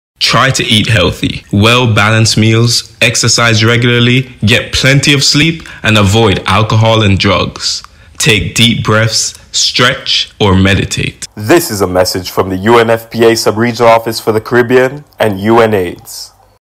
COVID19: AUDIO message on People Living with HIV : Supporting you and those around you –Eat healthy
Try to eat healthy, well-balanced meals, exercise regularly, get plenty of sleep and avoid alcohol and drugs. Take deep breaths, stretch or meditate.
This is a message from the UNFPA Sub-Regional Office for the Caribbean and our sister agency UNAIDS Jamaica.